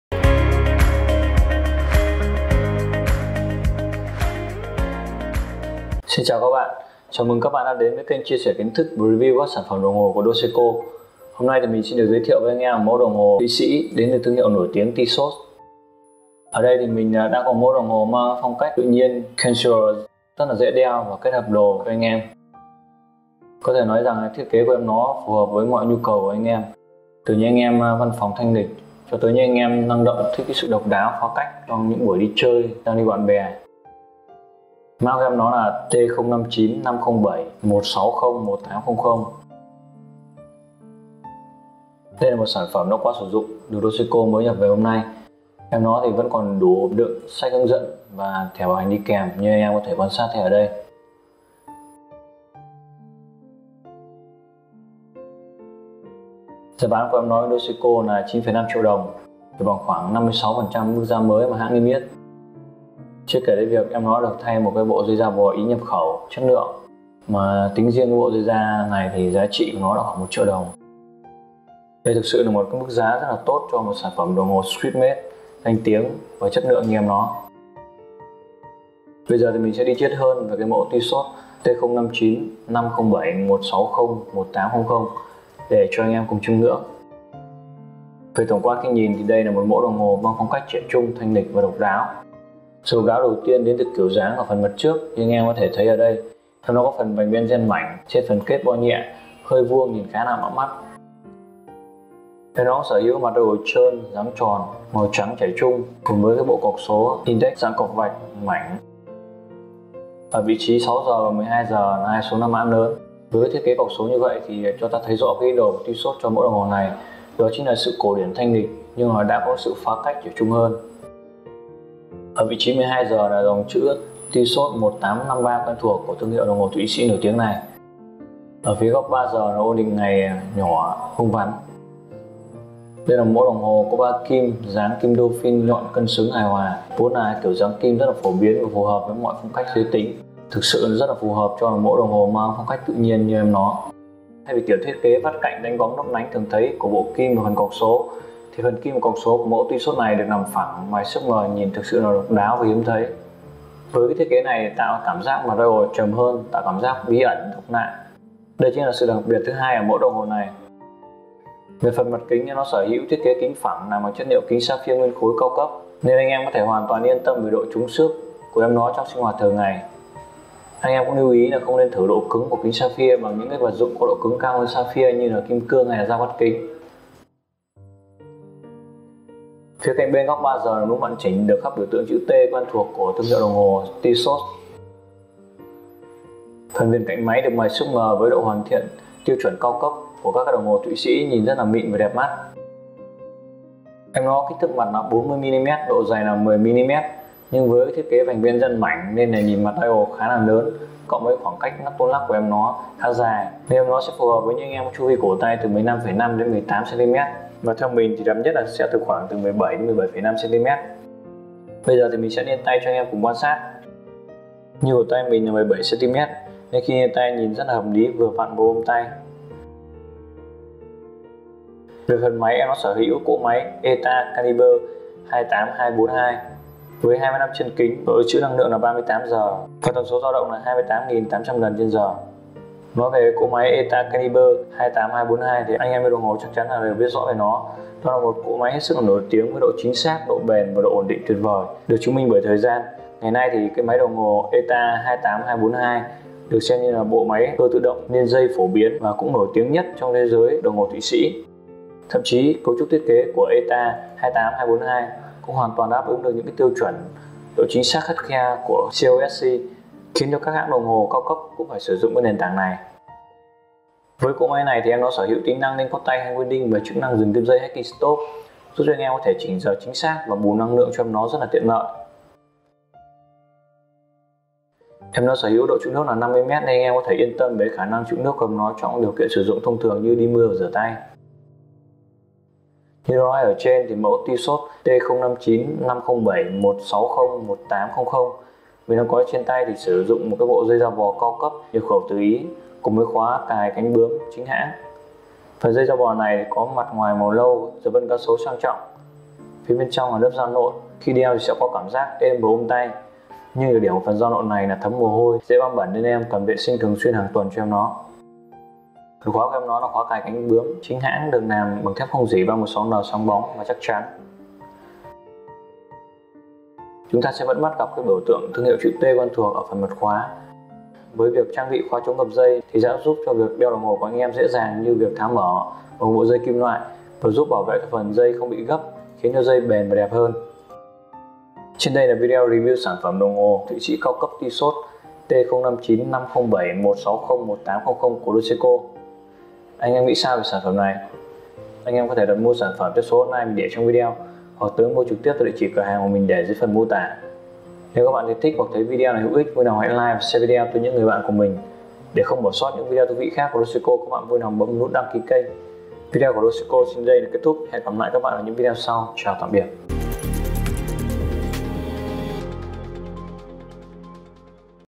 Review đồng hồ #15: Tissot T-lord T059.507.16.018.00 – Men's Casual Watches
►Xem giá bán sản phẩm tại đây: Hôm nay, DOSECO xin được giới thiệu với anh em một mẫu đồng hồ thụy sĩ đến từ thương hiệu nổi tiếng Tissot. Mã của em nó là T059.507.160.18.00.
Đây là một mẫu đồng hồ mang phong cách tự nhiên Casual  rất dễ đeo và kết hợp đồ cho anh em. Có thể nói rằng thiết kế của em nó phù hợp với mọi nhu cầu của anh em, từ những anh em văn phòng thanh lịch cho tới những anh em năng động thích sự độc đáo, phá cách trong những buổi đi chơi giao lưu bạn bè.

Thông số tham khảo:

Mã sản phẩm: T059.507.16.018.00
Thương hiệu: Tissot
Xuất xứ: Thụy sĩ
Loại máy: Automatic
Phong cách: Casual
Giới tính: Nam
Kính Mặt: Sapphia
Kiểu dáng: Mặt tròn
Màu mặt: Trắng
Vỏ: Thép không gỉ 316L
Dây: Da bò Ý nhập khẩu dập vân cá sấu
Đường kính mặt:  40 mm
Độ dày: 10 mm
Độ chịu nước: 50m